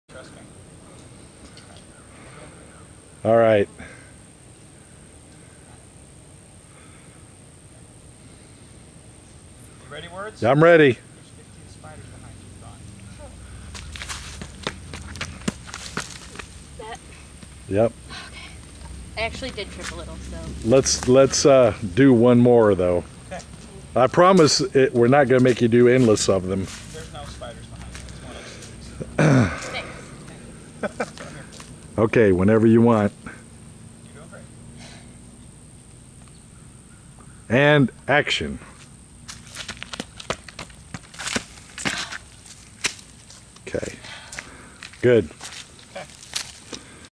No Budget Productions Behind the Scenes.... THE HILL running and fall
No Budget Productions Behind the Scenes. THE HILL running and fall from the star trek fan film